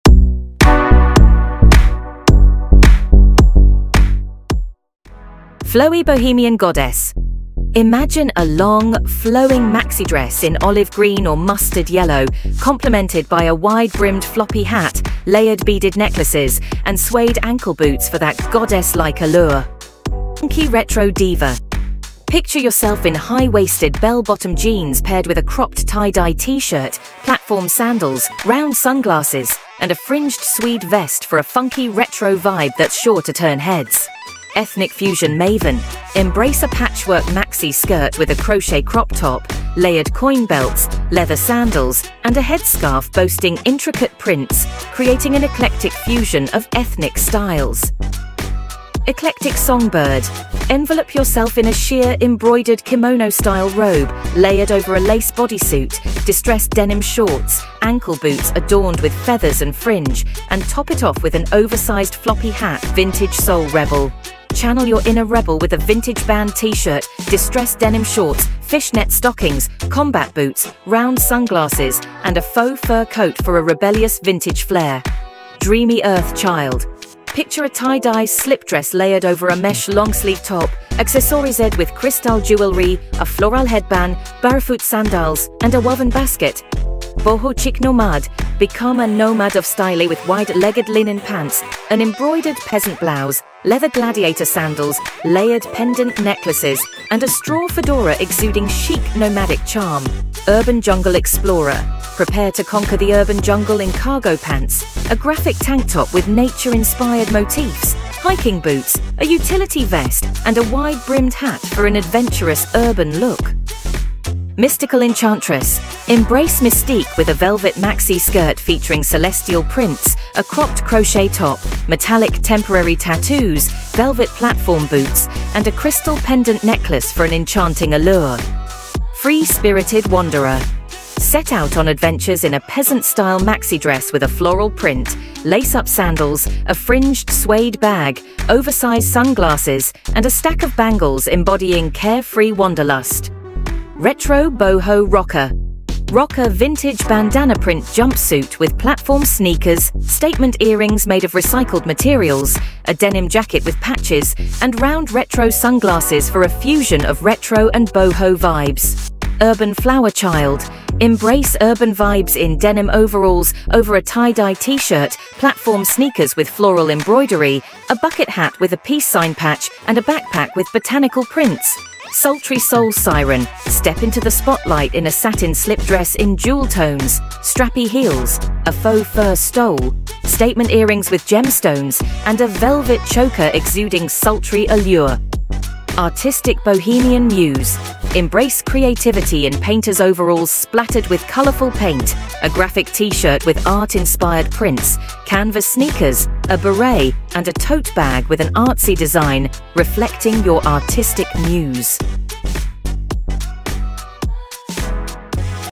How to Find Hippie Neo Soul Outfits to Try on Your Next Outing
Flowy Bohemian Goddess: Imagine a long, flowing maxi dress in olive green or mustard yellow, complemented by a wide-brimmed floppy hat, layered beaded necklaces, and suede ankle boots for that goddess-like allure.

 Funky Retro Diva: Picture yourself in high-waisted bell-bottom jeans paired with a cropped tie-dye t-shirt, platform sandals, round sunglasses, and a fringed suede vest for a funky retro vibe that's sure to turn heads.

 Ethnic Fusion Maven: Embrace a patchwork maxi skirt with a crochet crop top, layered coin belts, leather sandals, and a headscarf boasting intricate prints, creating an eclectic fusion of ethnic styles.

 Eclectic Songbird: Envelop yourself in a sheer, embroidered kimono-style robe layered over a lace bodysuit, distressed denim shorts, ankle boots adorned with feathers and fringe, and top it off with an oversized floppy hat for a bohemian rocker edge.

 Vintage Soul Rebel:  Channel your inner rebel with a vintage band t-shirt, distressed denim shorts, fishnet stockings, combat boots, round sunglasses, and a faux fur coat for a rebellious vintage flair.

 Dreamy Earth Child:  Picture a tie-dye slip dress layered over a mesh long-sleeve top, accessorized with crystal jewelry, a floral headband, barefoot sandals, and a woven basket bag for an enchanting, earthy ensemble.

 Boho Chic Nomad:  Become a nomad of style with wide-legged linen pants, an embroidered peasant blouse, leather gladiator sandals, layered pendant necklaces, and a straw fedora exuding chic nomadic charm.

 Urban Jungle Explorer: Prepare to conquer the urban jungle in cargo pants, a graphic tank top with nature-inspired motifs, hiking boots, a utility vest, and a wide-brimmed hat for an adventurous urban look.

 Mystical Enchantress: Embrace mystique with a velvet maxi skirt featuring celestial prints, a cropped crochet top, metallic temporary tattoos, velvet platform boots, and a crystal pendant necklace for an enchanting allure.

Free-spirited Wanderer: Set out on adventures in a peasant-style maxi dress with a floral print, lace-up sandals, a fringed suede bag, oversized sunglasses, and a stack of bangles embodying carefree wanderlust.

Retro Boho Rocker: Rock a vintage bandana-print jumpsuit with platform sneakers, statement earrings made of recycled materials, a denim jacket with patches, and round retro sunglasses for a fusion of retro and boho vibes.

Urban Flower Child: Embrace urban vibes in denim overalls over a tie-dye t-shirt, platform sneakers with floral embroidery, a bucket hat with a peace sign patch, and a backpack with botanical prints.

Sultry Soul Siren: Step into the spotlight in a satin slip dress in jewel tones, strappy heels, a faux fur stole, statement earrings with gemstones, and a velvet choker exuding sultry allure.

Artistic Bohemian Muse: Embrace creativity in painter's overalls splattered with colorful paint, a graphic t-shirt with art-inspired prints, canvas sneakers, a beret, and a tote bag with an artsy design reflecting your artistic muse.

These outfits blend bohemian fashion with a neo soul vibe, creating unique and expressive looks that speak to your individuality and adventurous spirit.